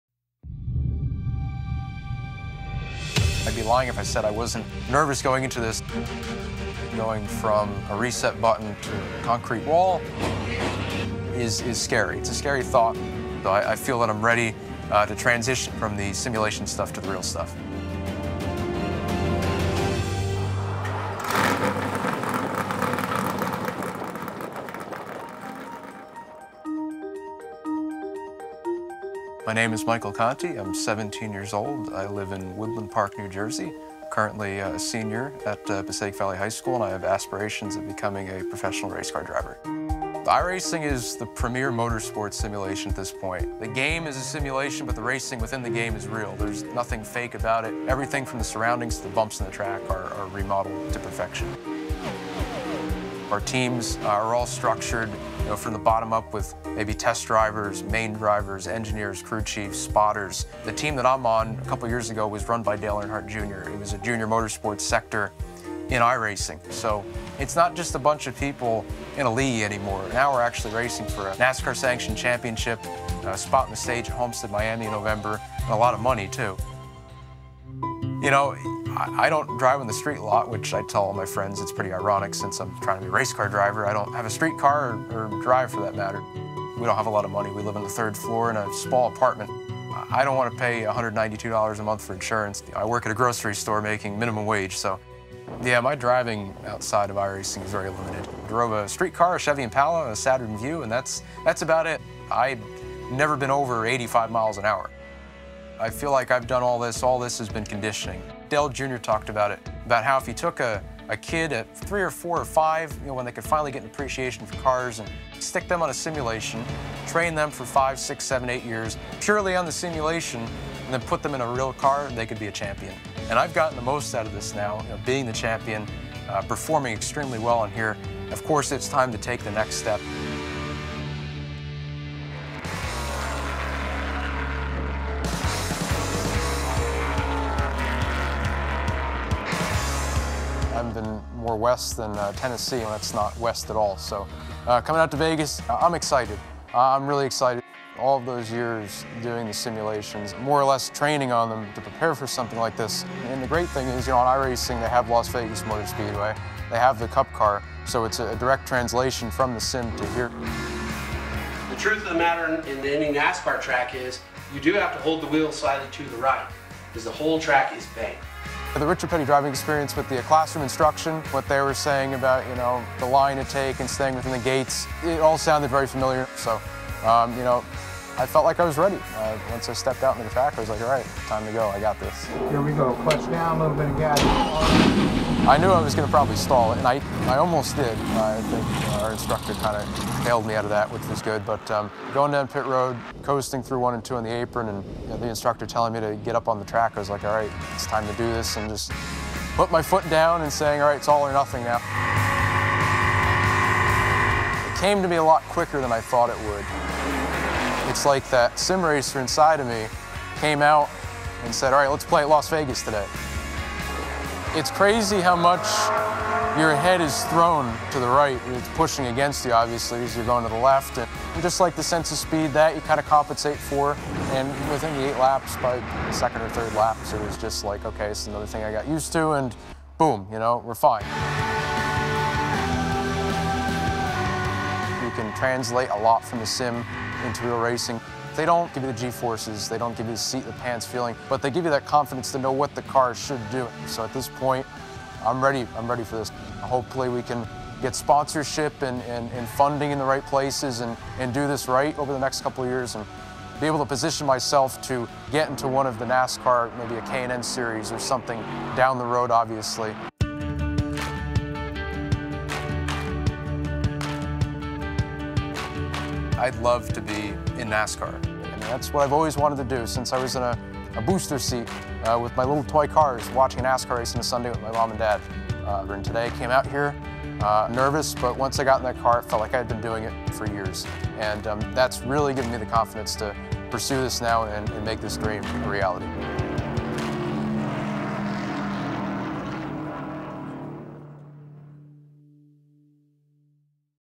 iRacing vs. NASCAR: A 17-year-old sim racer's first real drive
Michael Conti is a 17-year-old high school student from N.J. with aspirations of becoming a professional race car driver. In many ways, Conti has achieved part of his dream, as he is the 2014 NASCAR PEAK Antifreeze Series champion on the online racing sim, iRacing. But while handy on a computer, Conti has very little real-world driving experience, and the fastest he has ever driven is 85 mph in his dad's 2003 Chevy Impala. That is, until Yahoo Autos sent him to Las Vegas Motor Speedway and put him behind the wheel of an actual 600+hp stock car.